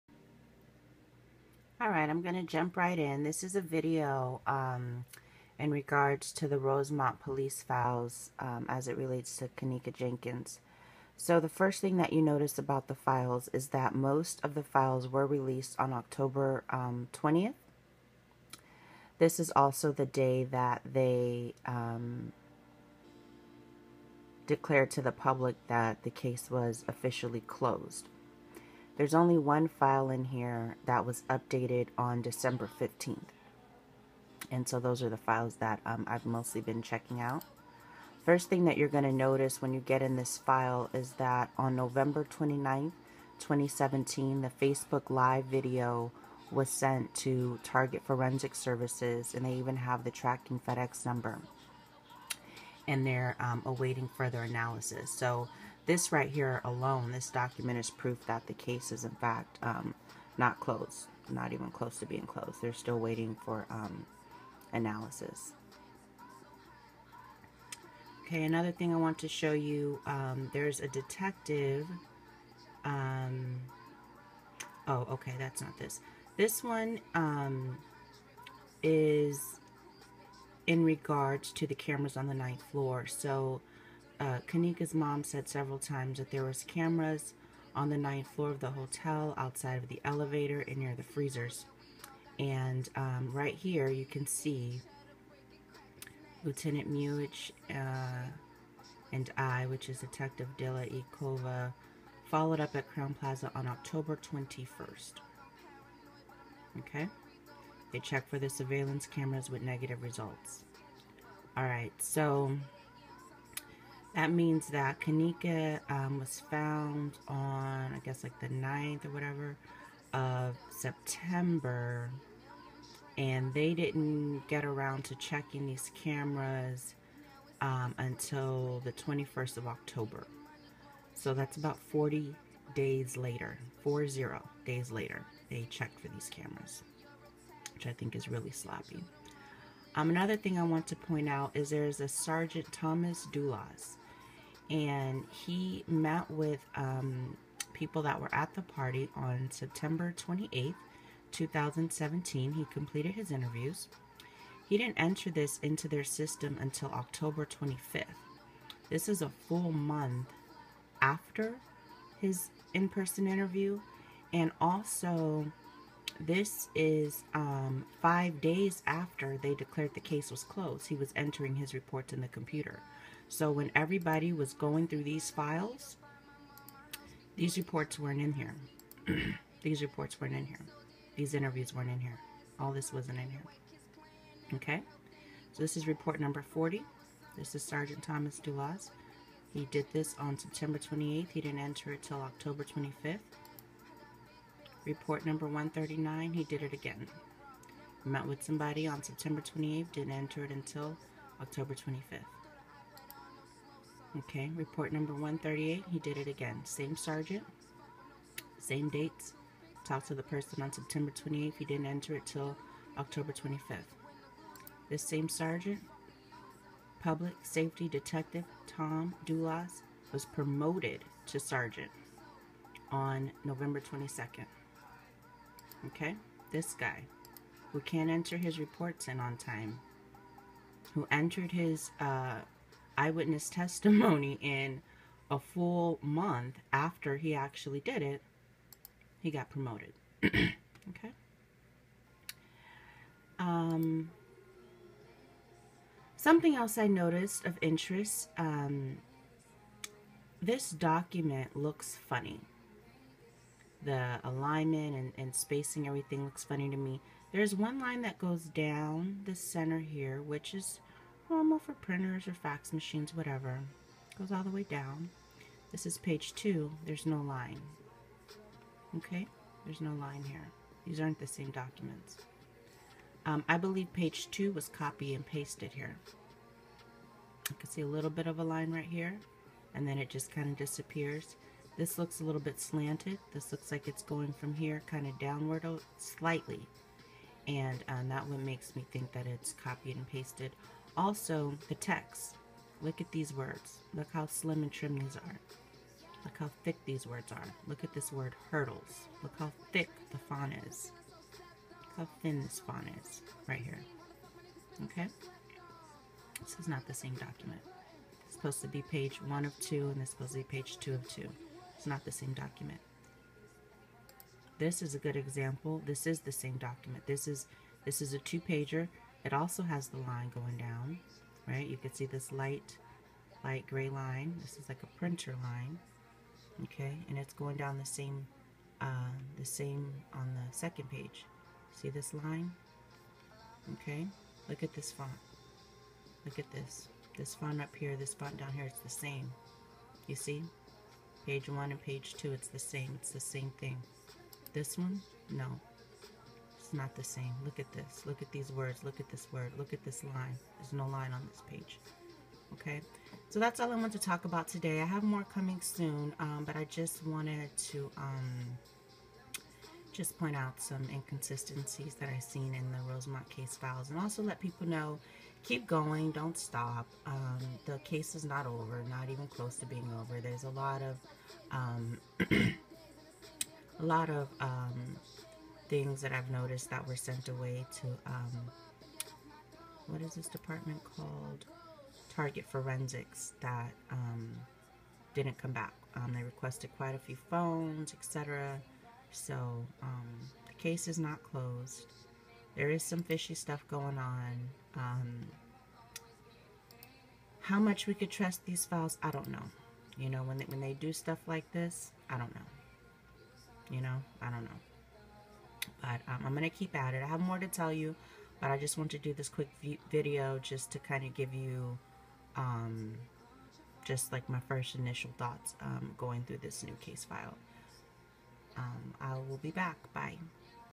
Kenneka Jenkins case still open- late reports and document fraud???
What's in the Rosemont files?

1. Proof the case is still open: Rosemont PD sent evidence to forensics on November 29, 2017 nearly 40 days AFTER they said the case was closed.

2. Rosemont police didn't hunt down the cameras on the 9th floor and above the freezer until October 21, 2017 nearly 40 days AFTER Kenneka was found dead in the hotel. By this time, the cameras that Kenneka's mother was adamant about witnessing were gone.

3. Sgt. Doulas and his late reports can be found here- then just scroll to the right to find the other two

4. I don't know when Sgt. Doulas was promoted to sergeant, but it seems recent as he was just officially recognized and celebrated by his department on November 22, 2017. Seems suspect that such a good employee would enter his reports a full month LATE and 5 days AFTER the case was declared closed.

5. Here is the document that I suspect is fraud. Page 2 is suspect [This is what I believe the two page document should look like: notice the consistency in lines and font